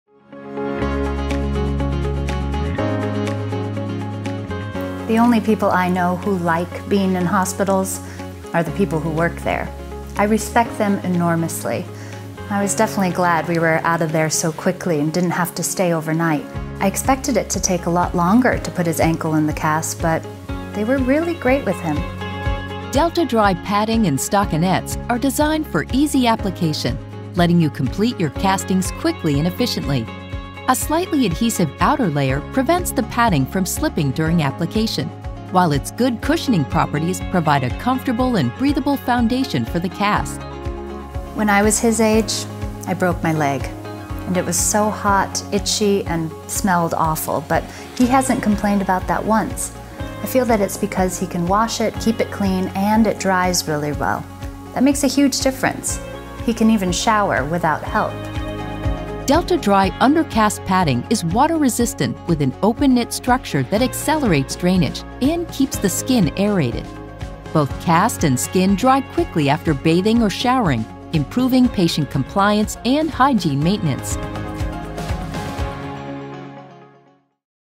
Delta-Dry® - benefits of a water-resistant cast in maintaining a child's hygiene despite a fracture
The Delta-Dry® product range enable patients to perform daily hygiene without restrictions. Better hygiene, increased patient comfort, and fewer unscheduled cast changes are the result.

As Delta-Dry products are allowed to get wet, patients´ can wash their hands or shower without using cast protectors or cast covers.

Delta-Dry products are used under synthetic cast tapes providing a fully water resistant immobilization unit for the conservative treatment of fractures. If physicians allow, patients are able to swim, shower or bath during fracture treatment.

Find out in this video how Delta-Dry® water resistant products helps a mother maintain her child’s cast hygiene while having a broken ankle.